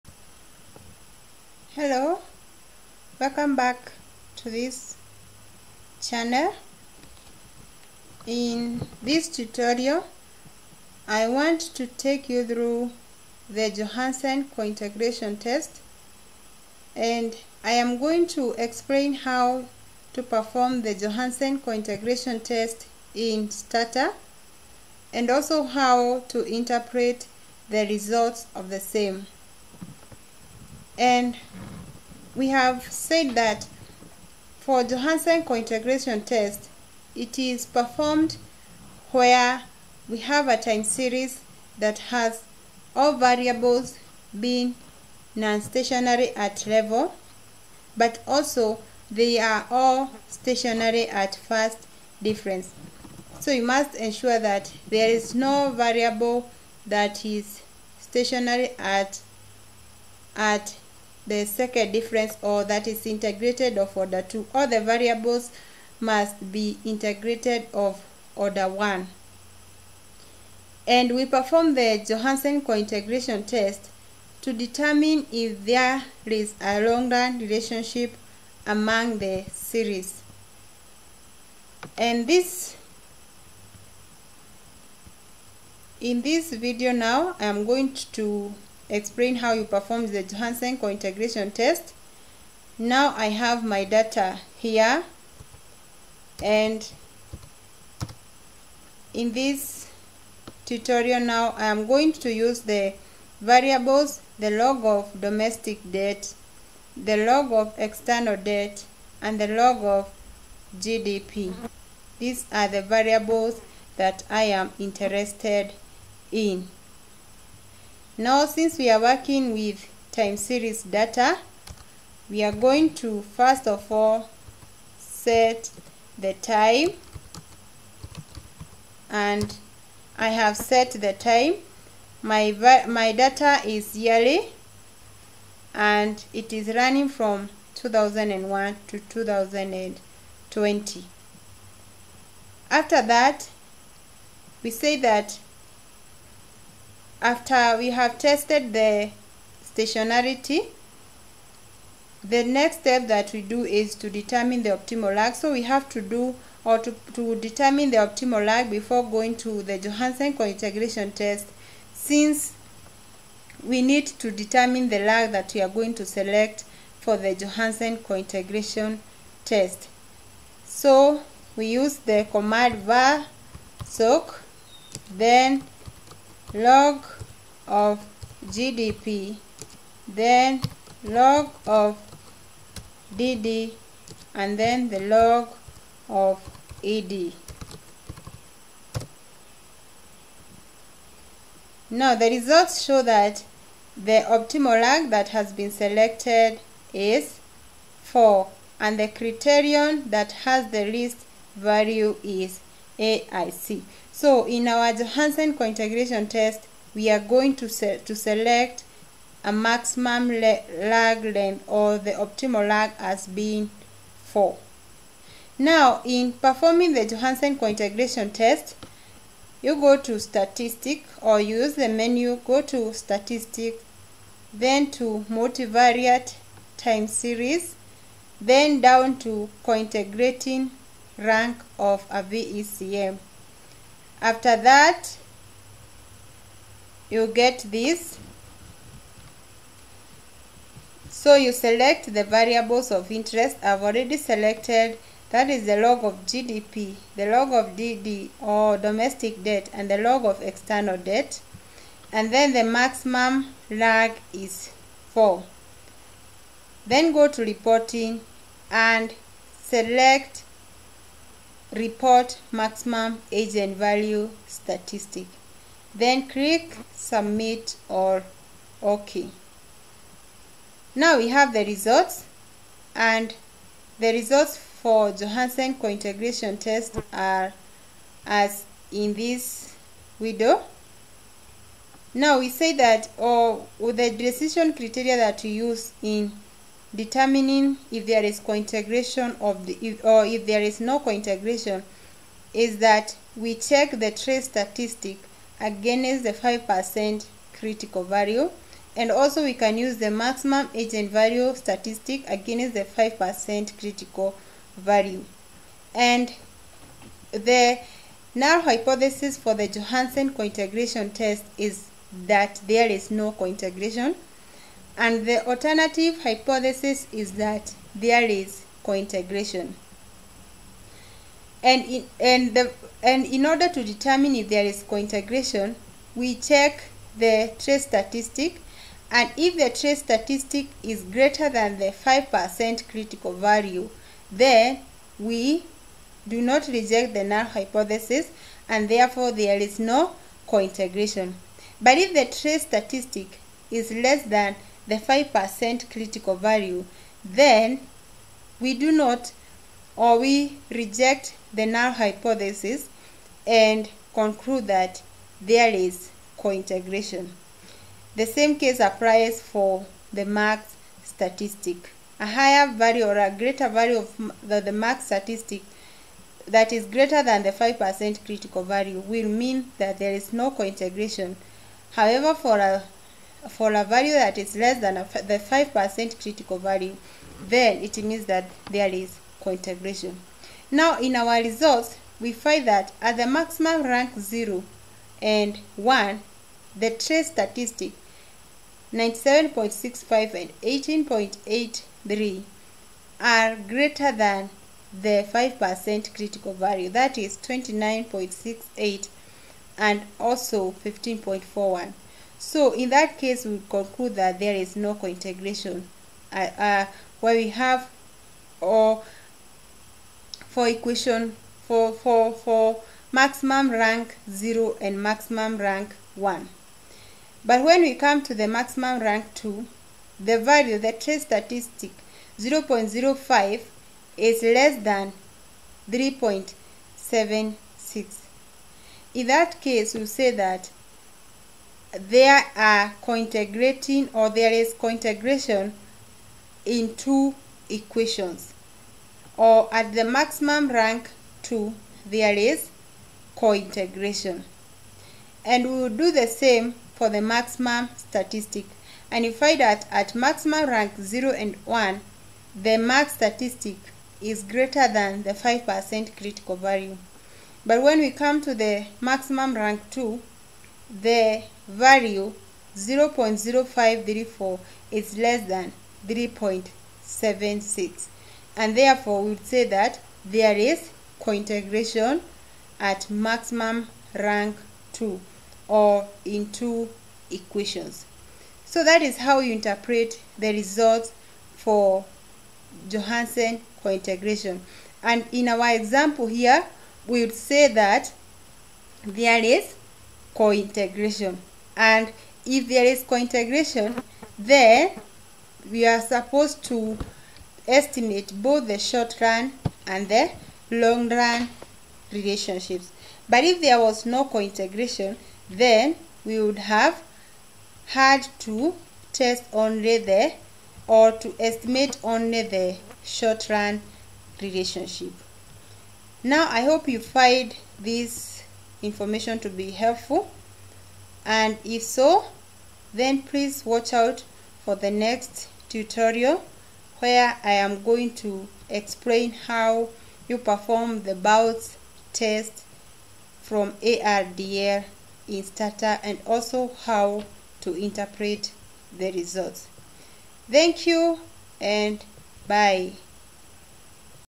Johansen Cointegration Test in STATA
In this tutorial, I take you through the procedure for performing the Johansen Cointegration Test in STATA. I also explain how to interpret the results in order to determine if there is cointegration.